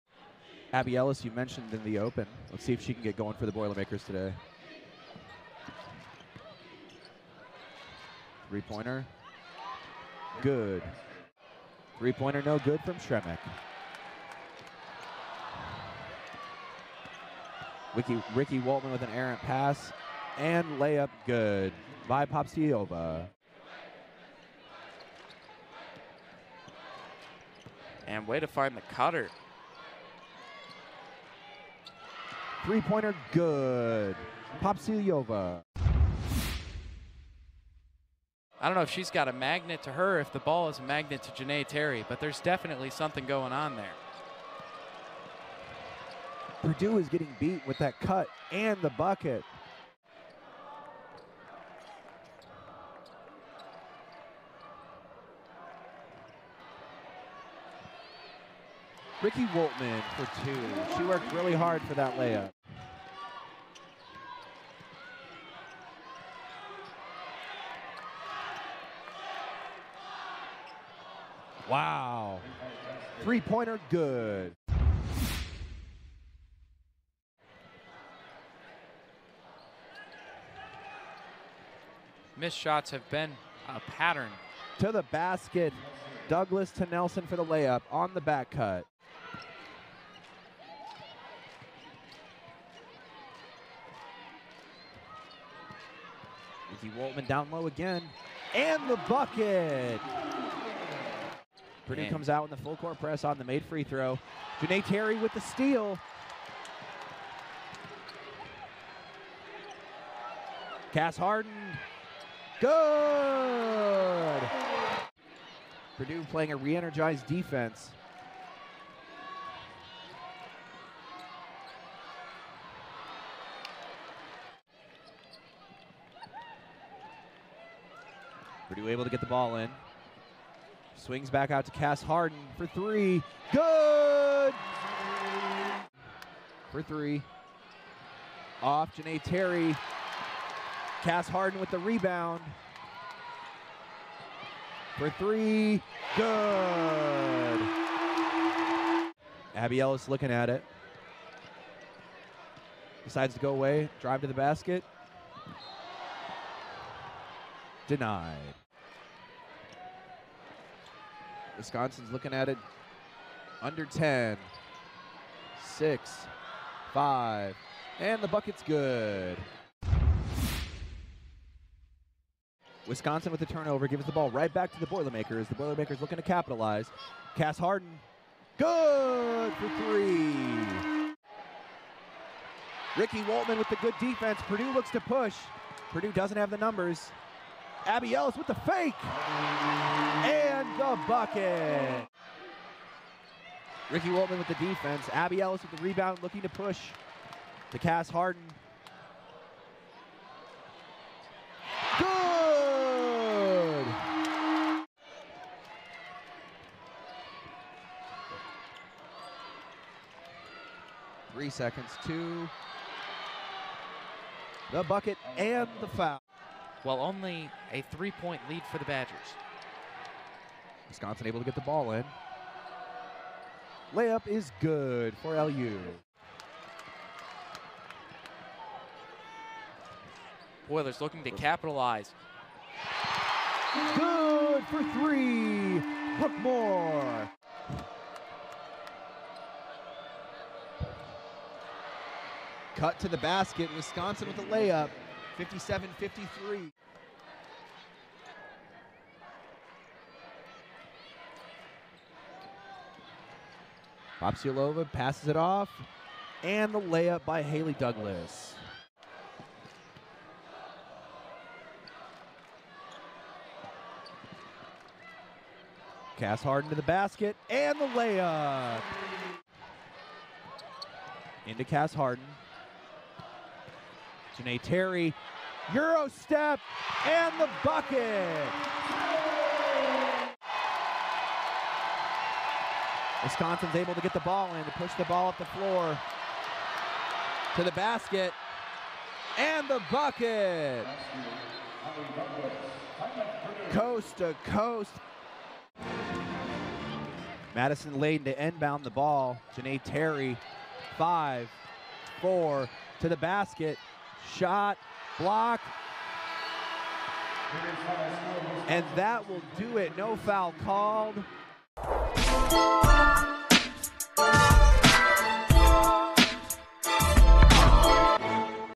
Wisconsin at Purdue | Highlights | Big Ten Women's Basketball | Feb. 27, 2022
Halle Douglass scored the game-winning basket to help lead Wisconsin to a 63-62 win over Purdue.